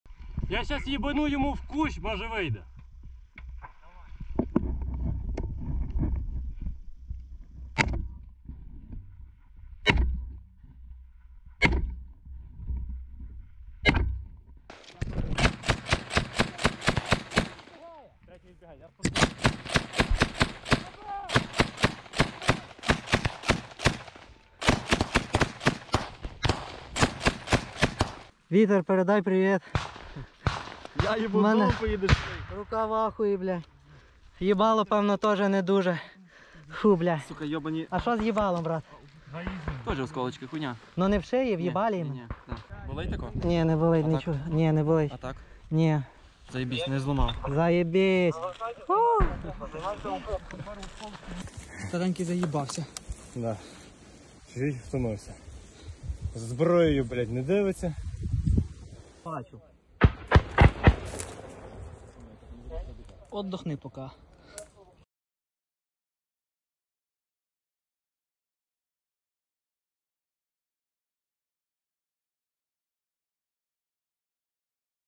Ukraine troops launch sniper assault on Russian soldiers as they capture enemy position
Footage from Ukraine's Third Separate Assault battalion shows them taking trenches near Klischivka, south of Bakhmut. The troops open fire with sniper rifles and machine guns before advancing and capturing the position. Ukraine is thought to have captured the town of Klischivka on 24 July.